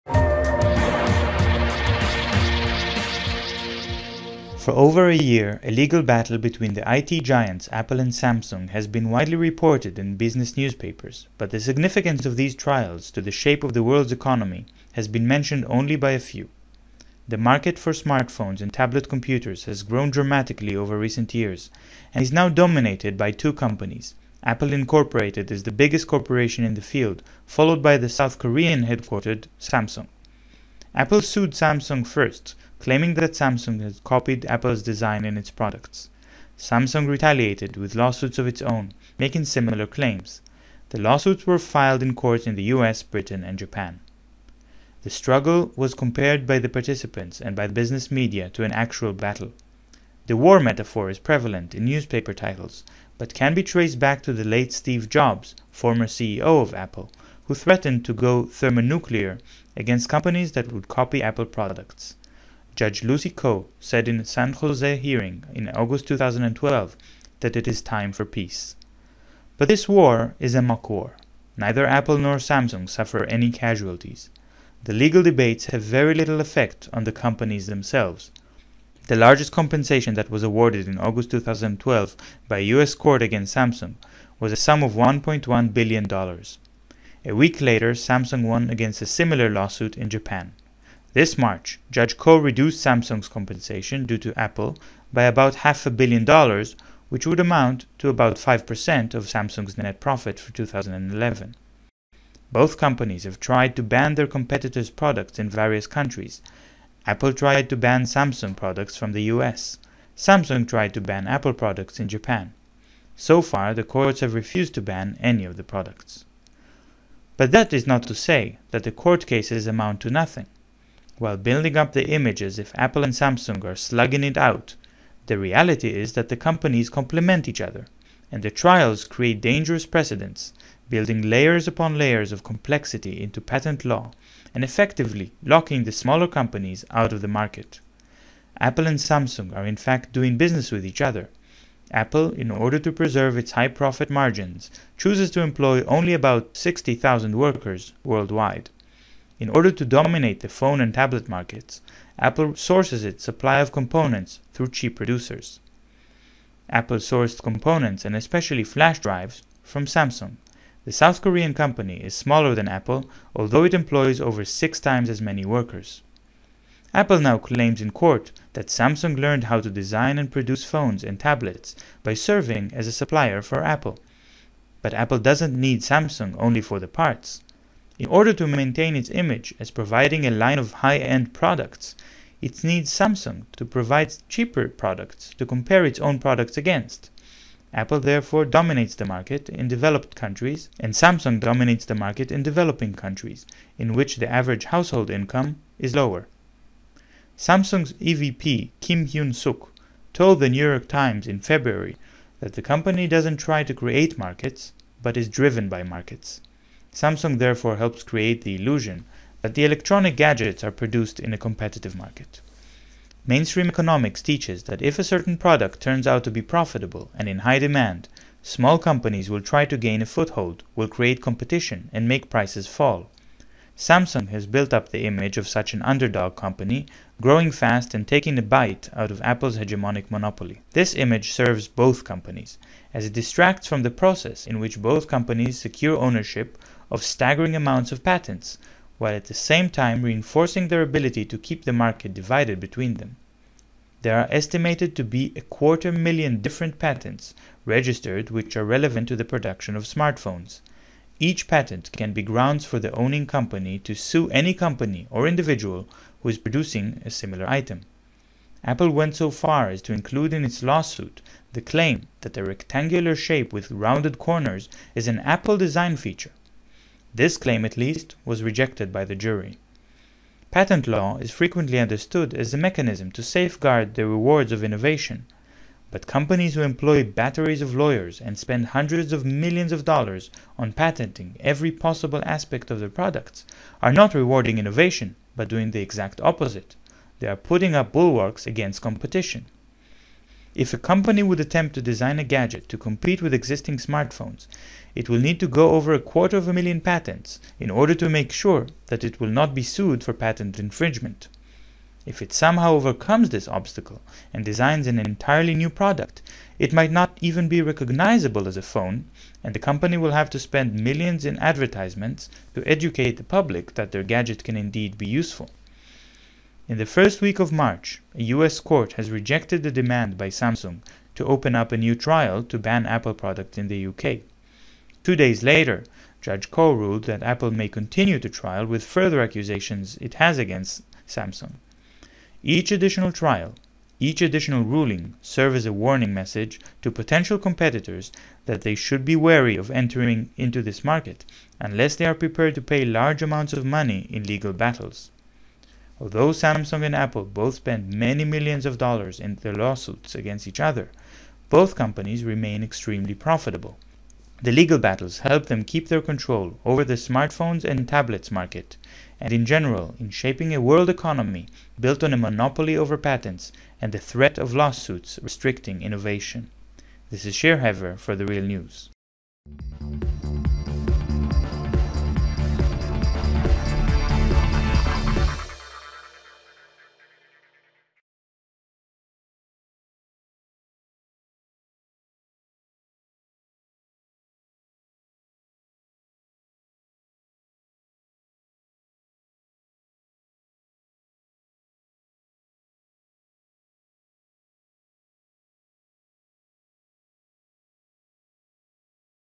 Mutual Interest Hidden Beneath Apple and Samsung Legal Struggle
Both companies benefit from turning patents into a bulwark against innovation, the general public pays the price when two corporations divide the market between them